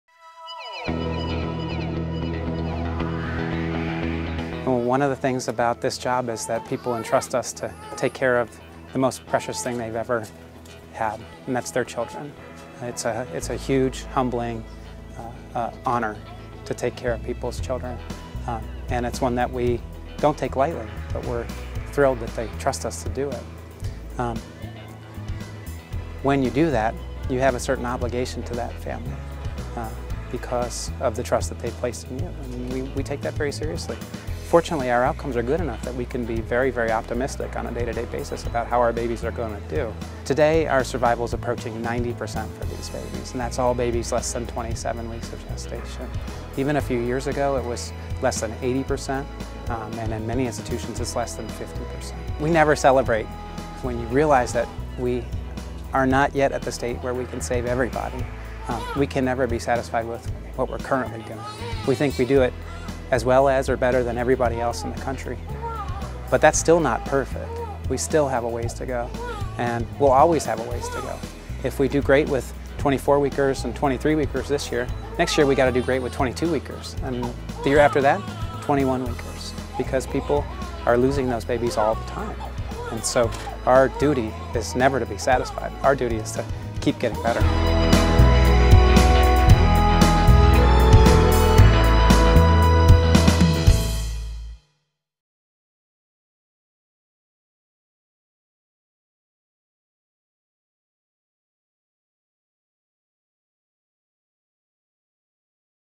Leading Neonatologist Discusses Survival Rates for Premature Babies
Outcomes are improving for premature babies born before 27 weeks gestation. Survival rate for these premature babies is 90 percent at Nationwide Children's Hospital. While survival rates are improving year-to-year, Dr. Ed Shepherd, section chief of neonatology at Nationwide Children's Hospital, is committed to constant improvement.